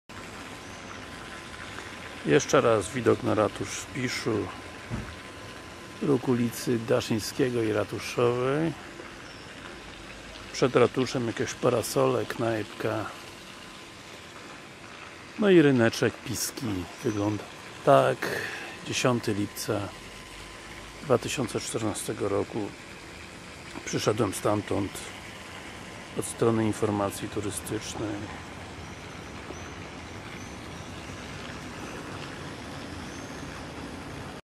Ratusz na rynku w Piszu
Kolejny etap wycieczki po Mazurach - Pisz. Ryneczek i ratusz robią wrażenie i widać ogrom pracy włożony w restaurację tych miejsc w centrum miasta. Róg Daszyńskiego i Ratuszowej